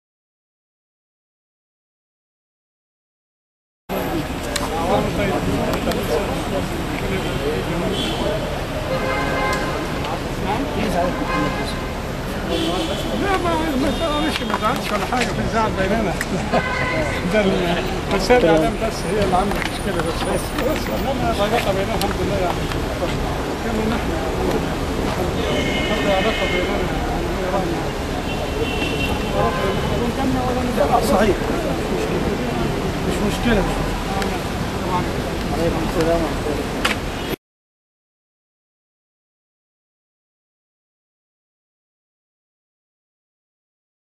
لحظة الخروج من قاعة الإفطار الجماعى المنظم من الإخوان
لحظة الخروج من قاعة الإفطار الجماعى المنظم من الإخوان المسلمين